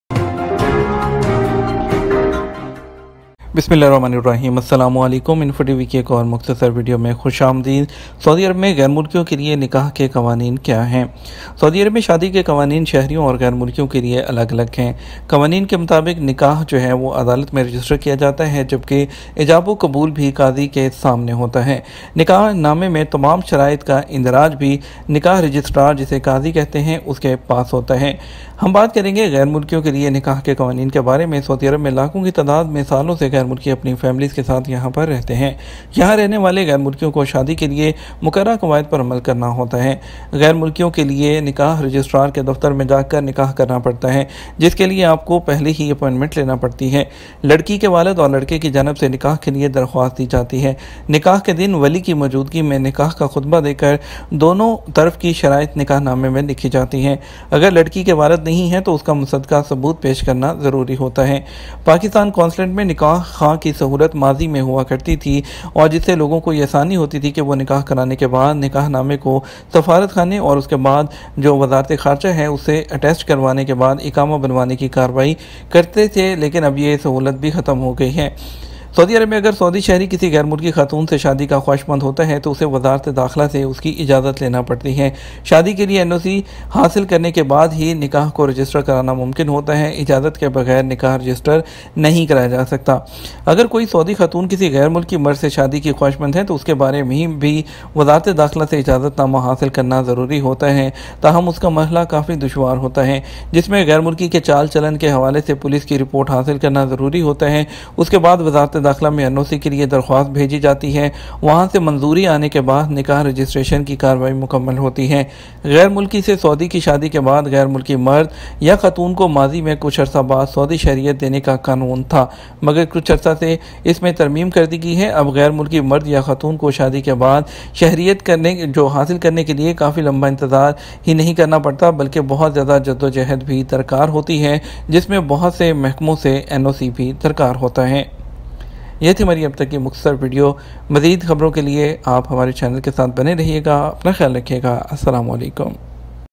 What are the Marriage Laws in Saudi Arabia For Expatriates in 2022 Urdu Hindi
Millions of foreigners have been living in the kingdom for years along with their families. Expatriates living here must follow the prescribed rules for marriage.
For foreigners, one has to go to the marriage registrar's office and get an appointment in advance. Application for Nikah is made by the father of the girl and the boy.
On the day of the marriage, in the presence of the guardian, the terms of the marriage sermon are recorded in the marriage certificate. If the girl does not have a father, then it is necessary to submit a certified proof.
In the past, there was a facility of Nikah Khawan in the Pakistan Consulate, which made it easy for people to get Iqama after getting the Nikah Nama confirmed by the Embassy and later by the Ministry of Foreign Affairs. has been done